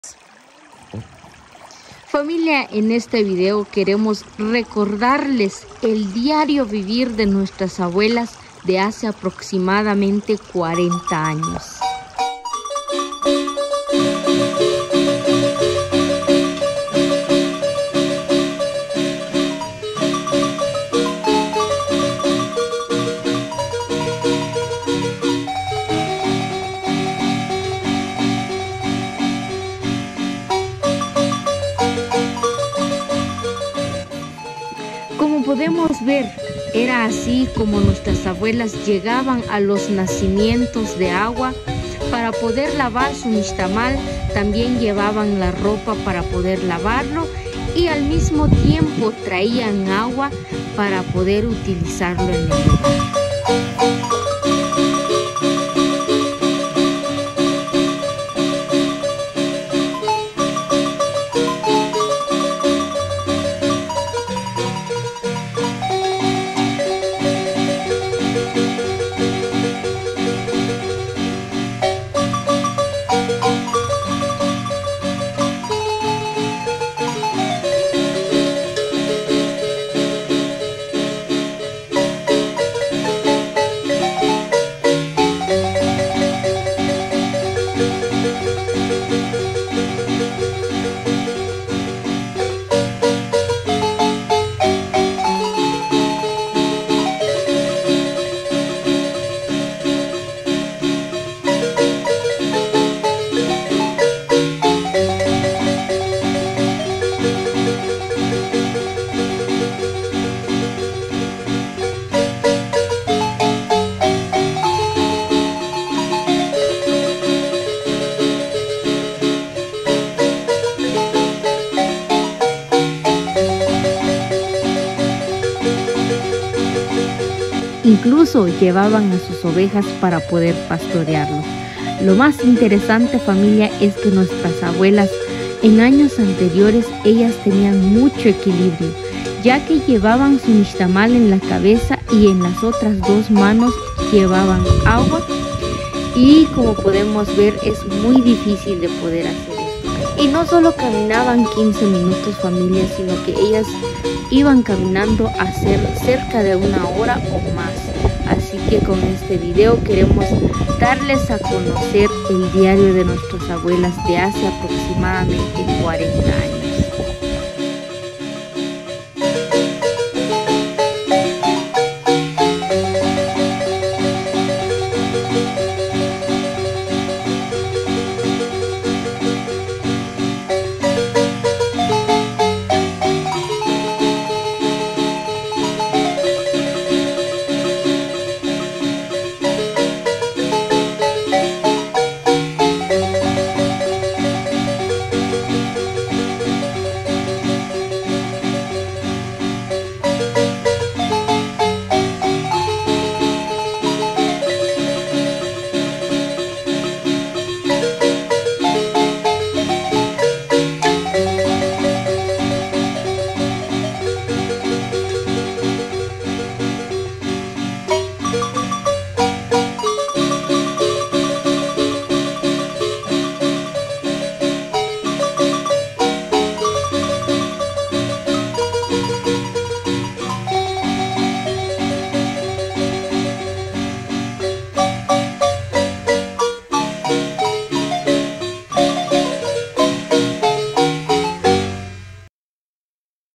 Nostálgica historia de la mujer Maya Mam Todosantera con Gladiz y Marty - Cuchumatán
Este video lo hacemos con la finalidad de recordar a nuestra abuelas de hace alrededor de 40 años en Todos Santos cuchumtán